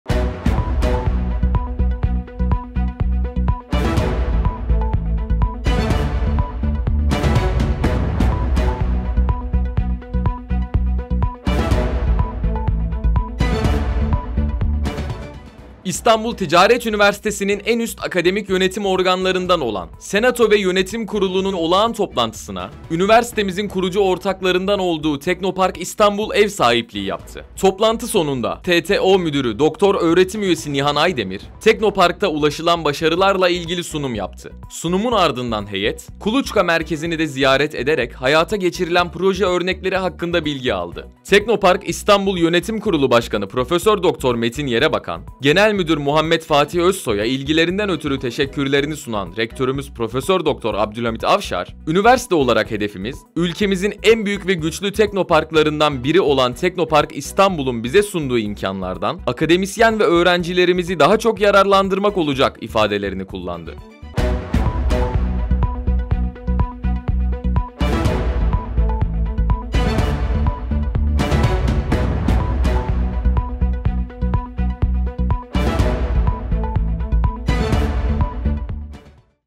Ticaret Medya Haber Bülteni - 04.06.2024
İstanbul Ticaret Üniversitesi'nin en üst akademik yönetim organları olan Senato ve Yönetim Kurulu’nun mayıs ayı olağan toplantısına, üniversitemizin kurucu ortaklarından olduğu Teknopark İstanbul ev sahipliği yaptı.

Toplantı sonunda Üniversitemiz TTO Müdürü Dr. Öğr. Ü. Nihan AYDEMİR, Teknopark’ta ulaşılan başarılarla ilgili sunum yaptı. Sunumun ardından heyet, Kuluçka Merkezini de ziyaret ederek hayata geçirilen proje örnekleri hakkında bilgi aldı.
Teknopark İstanbul YK Başkanı Prof. Dr. Metin Yerebakan, Genel Müdür Muhammet Fatih Özsoy'a ilgilerinden ötürü teşekkürlerini sunan Rektörümüz Prof. Dr. Abdulhamit Avşar, '' Üniversite olarak hedefimiz, ülkemizin en büyük ve güçlü teknoparklarından biri olan Teknopark İstanbul’un bize sunduğu imkanlardan akademisyen ve öğrencilerimizi daha çok yararlandırmak olacak'' ifadelerini kullandı.